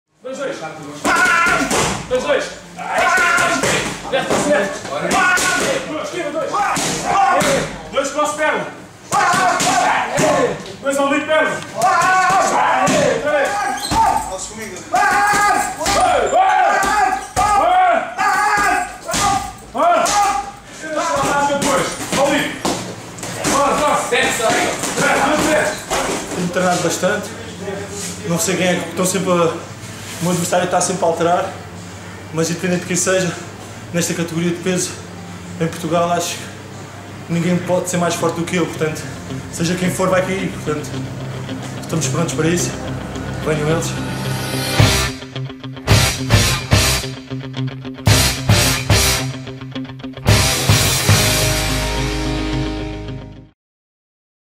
Pinto trailer 1
"Venham eles..." - Dia 8 de Março, na colectividade do Luso F.C., no Barreiro, esperamos por ti...